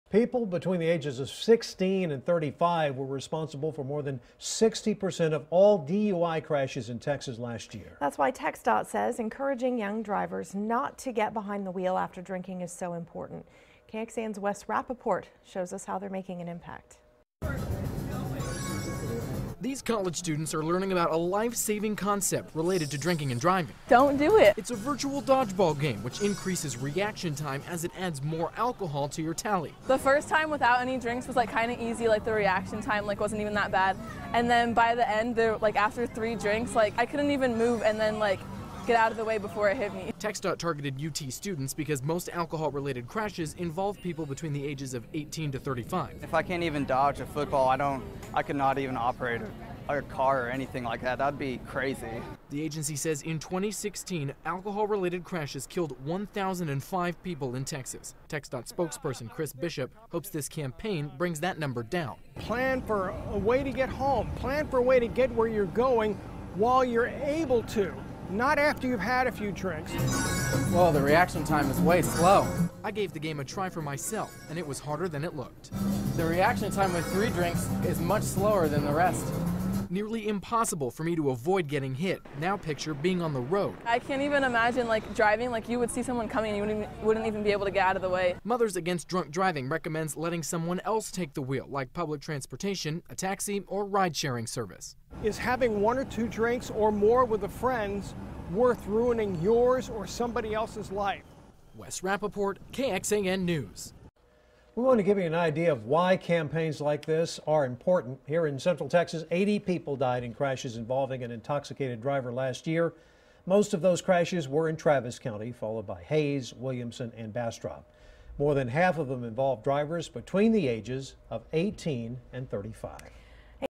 TxDOT uses virtual dodgeball game to send serious message about drunk driving
TxDOT reported 28 percent of crashes in Texas during 2016 were alcohol-related.